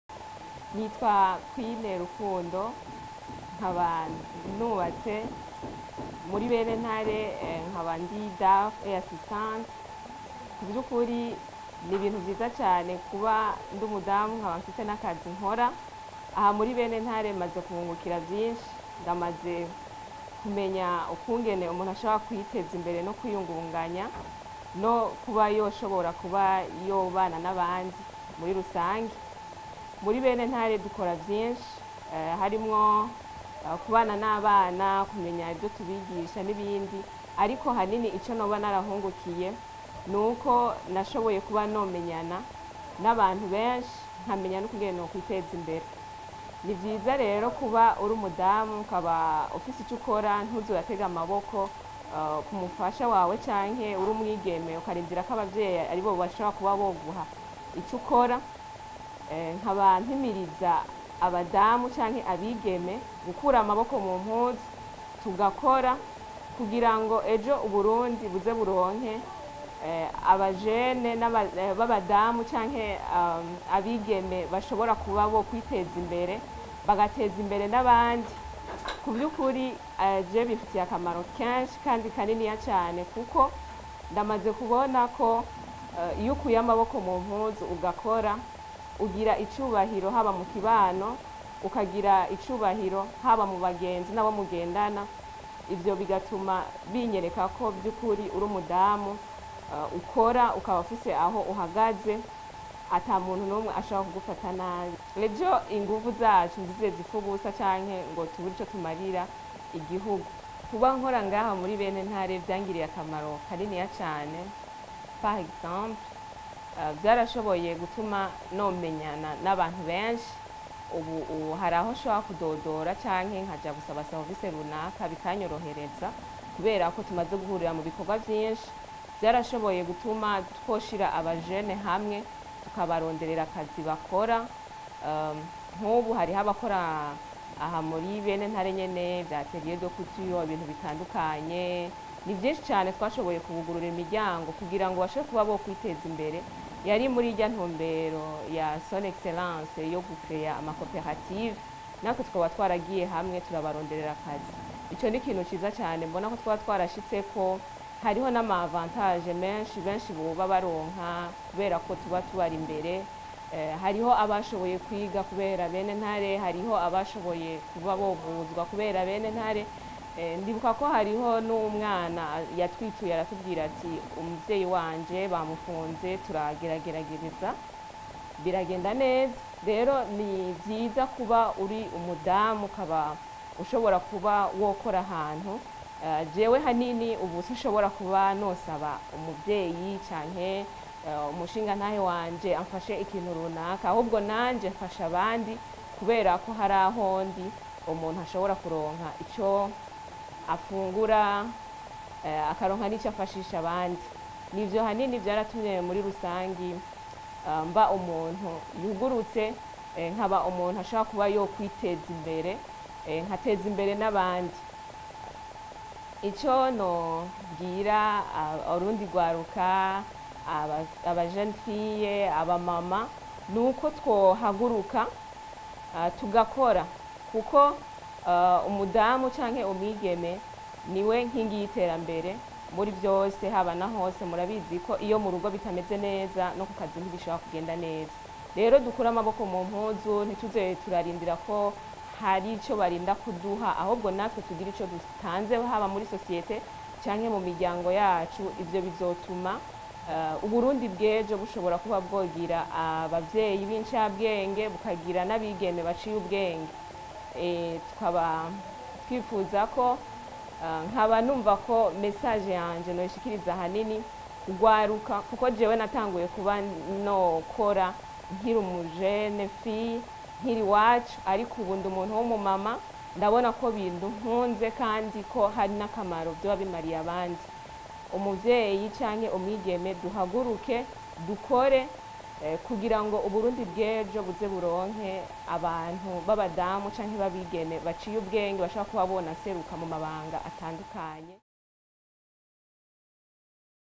Umupfasoni Queen Rukundo arahimiriza abigeme gutangura gukora bakiri bato ngo bashobore kwikenura.
Umukenyezi akwiriye kuba uwushigikira iterambere ry'urugo rwiwe n'iry'igihugu muri rusangi  kuko igihe co  kuzera inze cahise hageze ko abigeme n'abakenyezi bakura amaboko mu mpuzu bagakora bakiteza imbere.